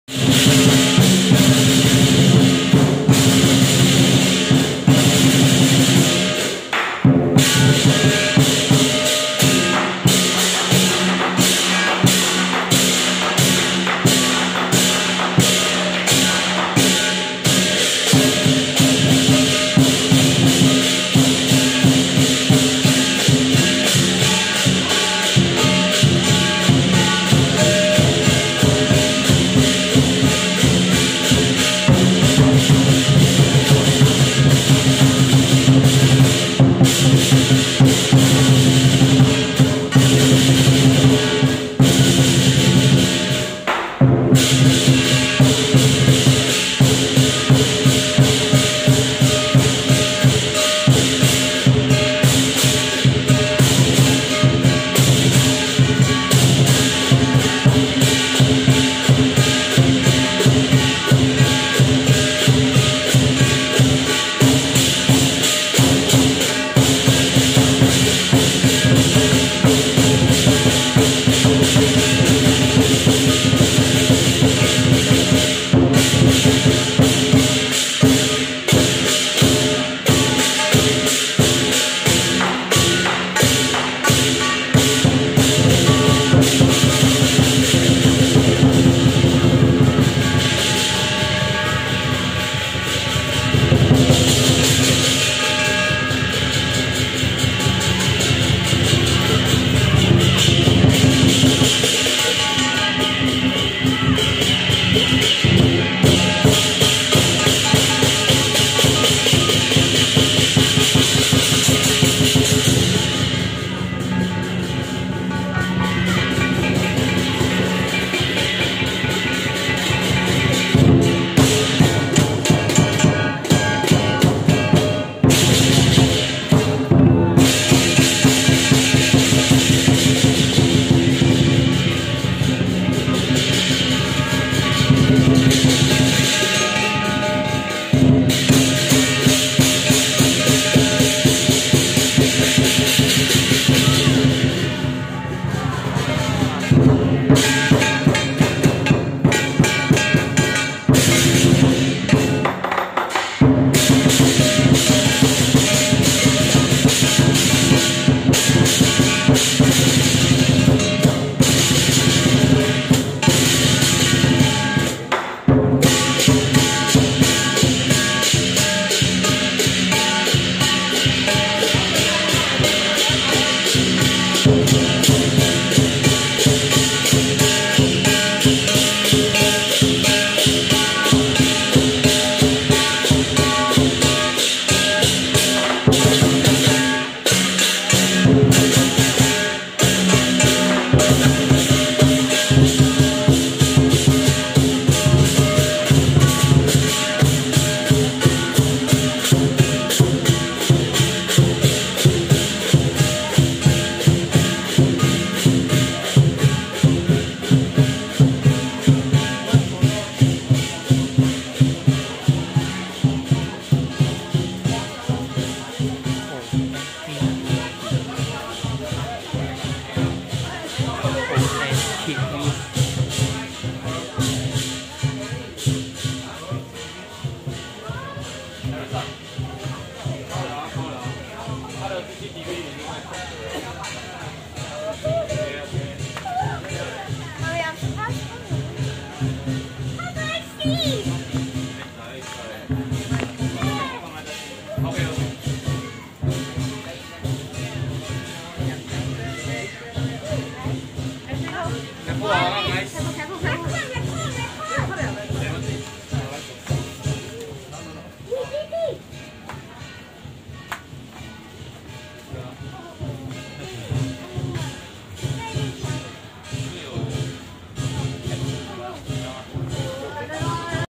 Choa Chu Kang Yew Tee MRT Station (NS5) Lion Dance Video Number 1
I Like The Lion Dance Video So That Everybody Can Enjoy. The Children Also Can Enjoyed The Lion Dance And Do The Lion Dance Performances At Home.